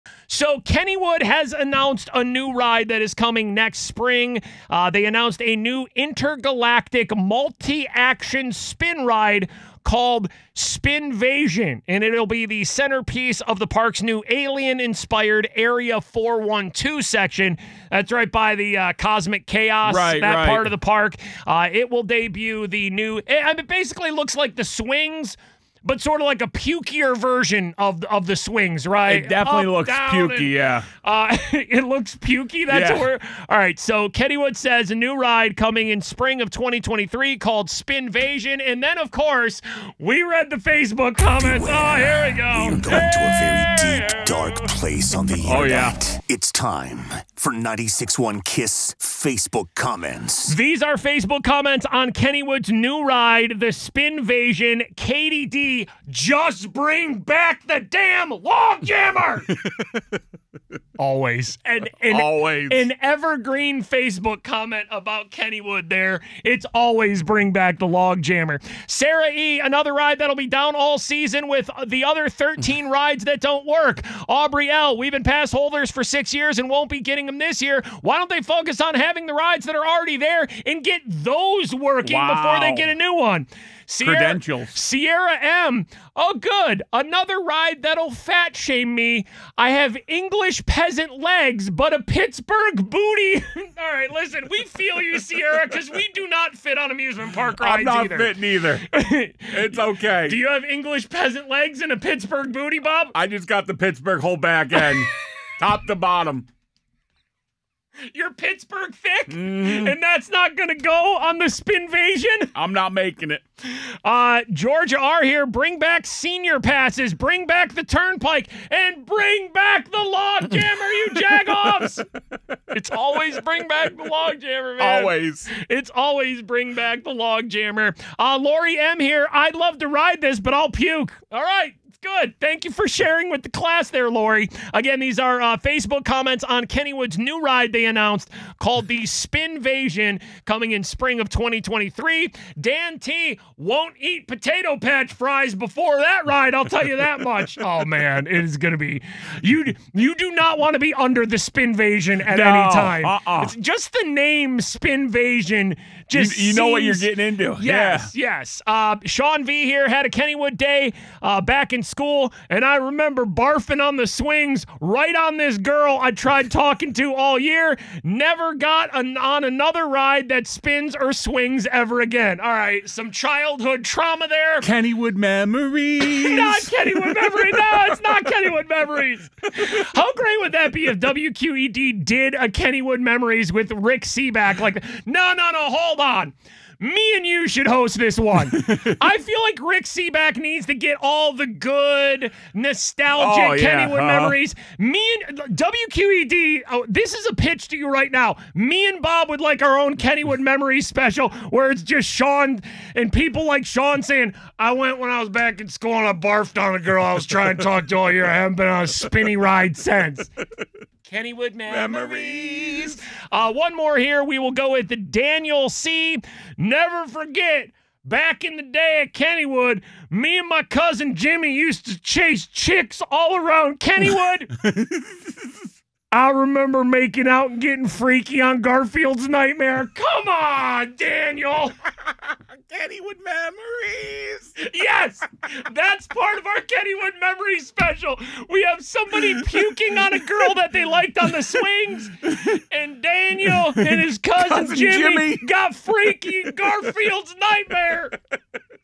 Spinvasion Kennywood ride Facebook comments
Whenever there's a new ride announced at Kennywood, the Facebook comments are ALWAYS incredible